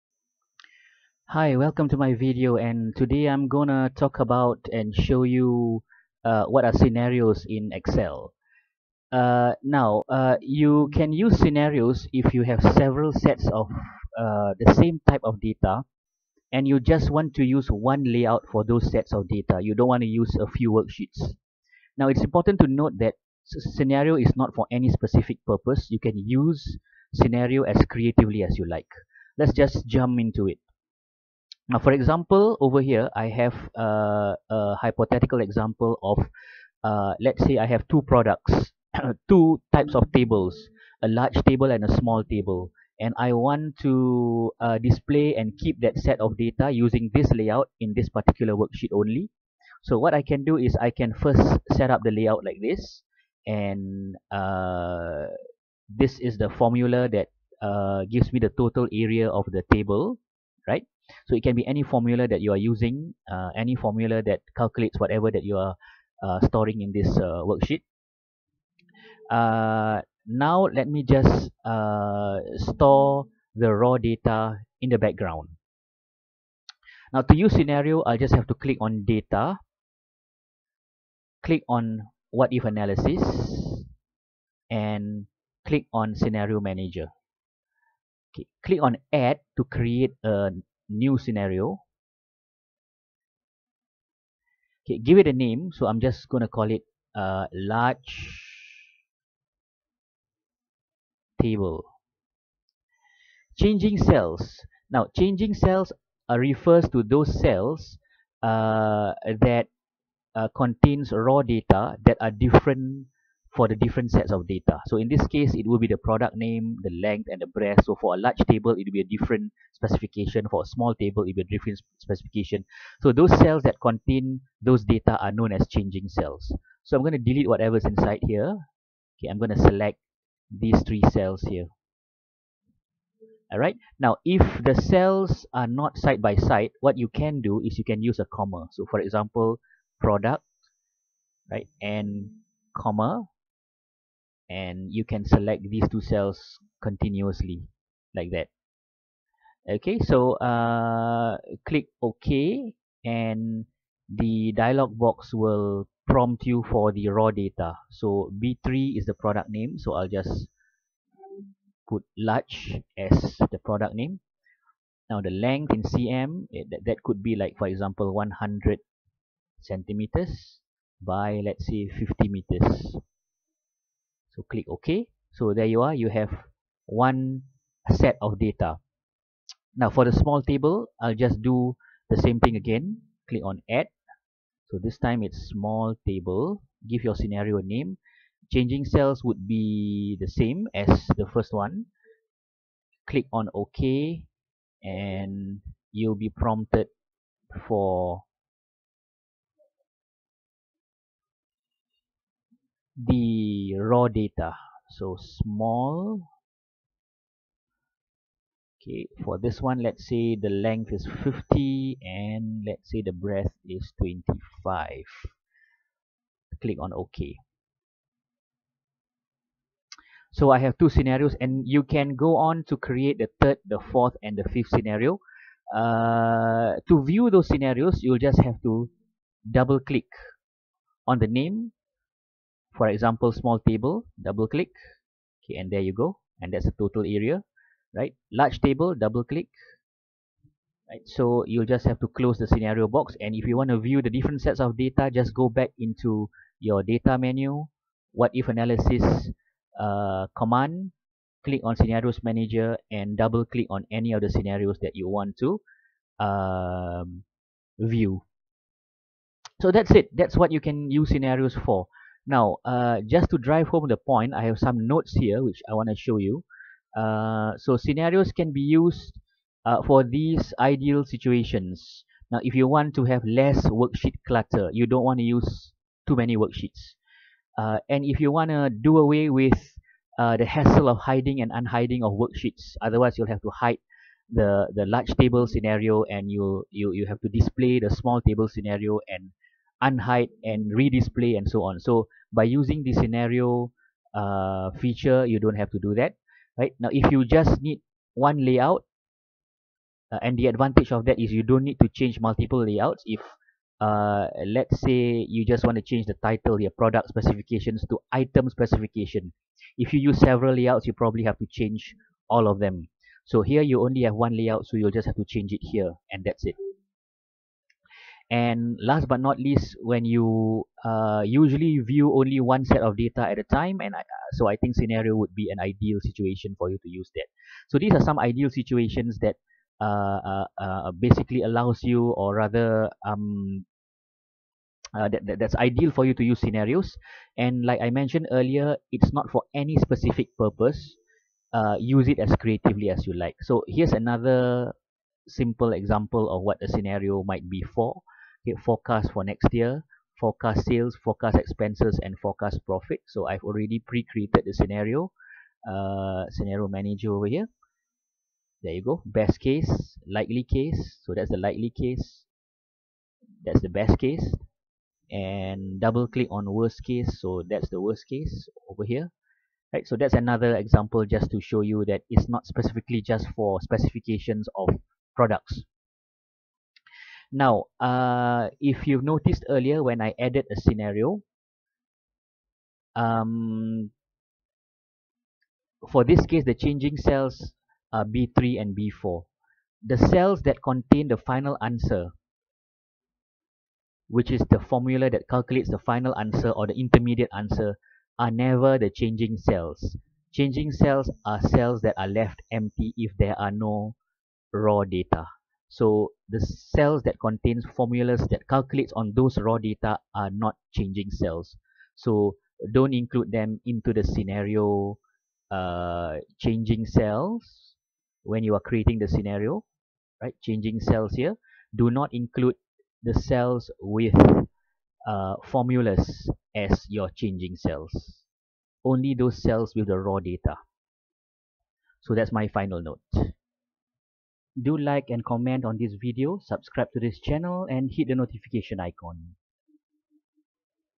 Scenarios - Creating and Showing Scenrios
Using the Scenarios feature of MS Excel spreadsheet, you can avoid some worksheet clutter or just have a single layout for several sets of data. There are other advantages as well. This video will show you how and why.